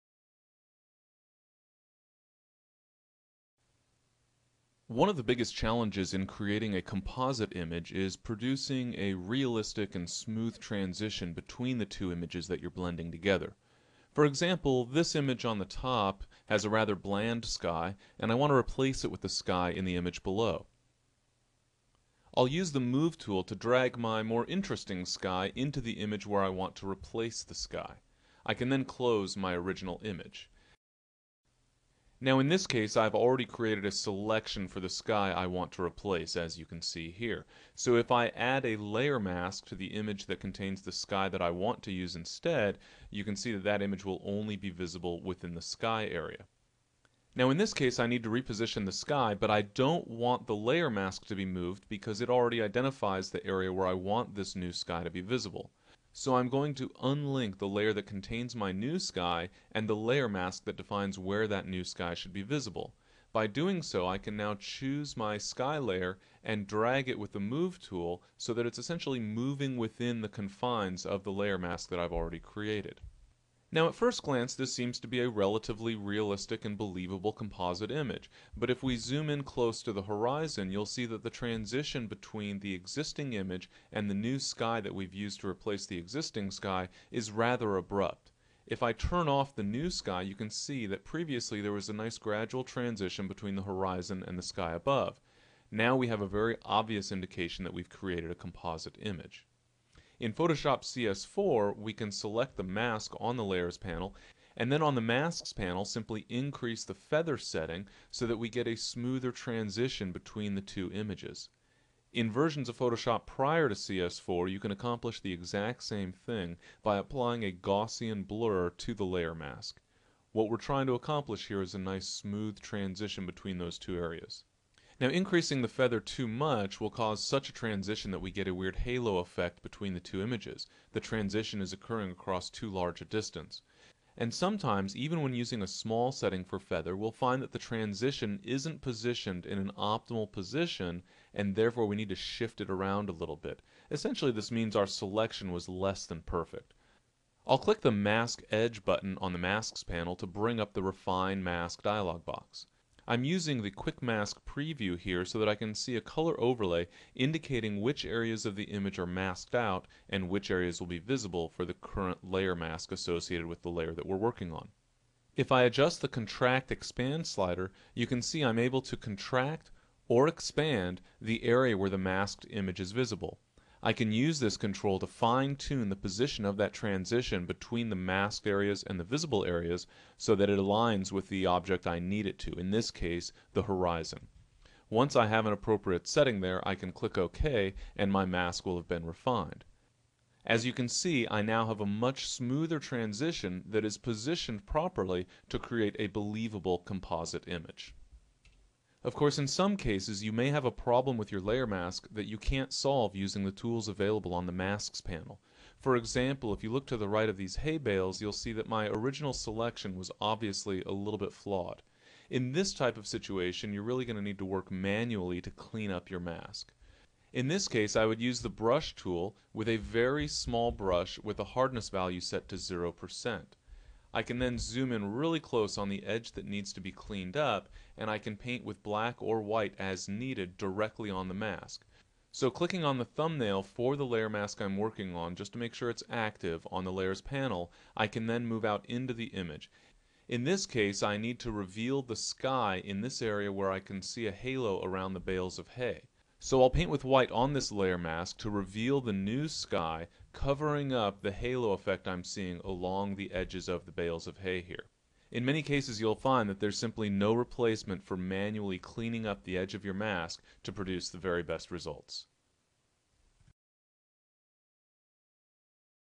Layer Mask Blending
Fine-tuning layer masks in Photoshop to produce the best results...